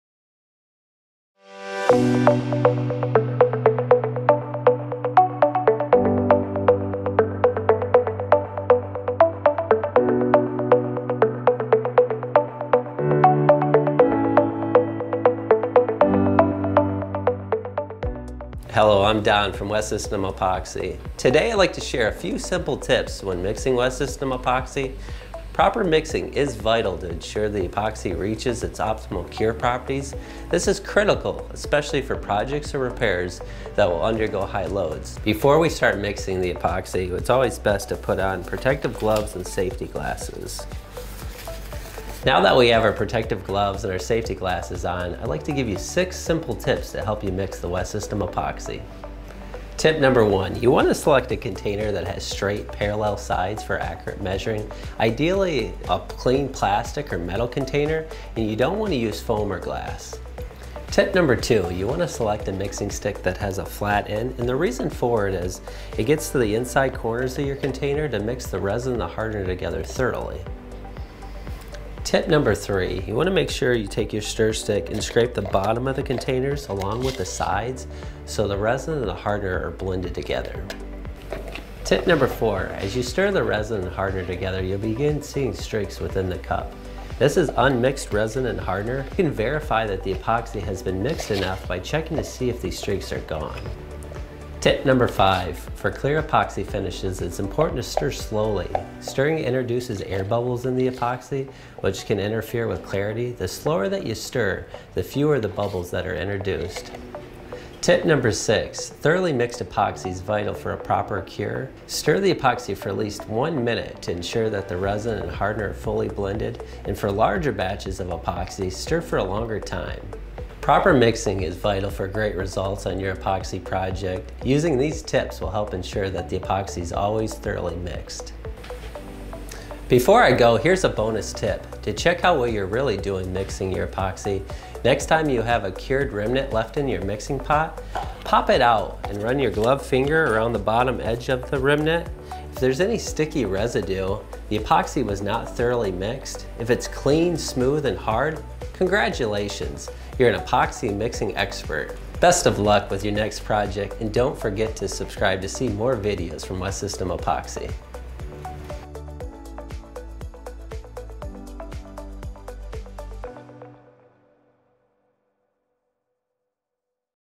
6 Easy Tips for Mixing WEST SYSTEM Epoxy
Fully mixed epoxy is crucial to success with any epoxy project. Following these six easy tips will help you be sure your epoxy is fully mixed before applying it to your project.

PRODUCTS USED
WEST SYSTEM 105 Resin

WEST SYSTEM User Manual and Product Guide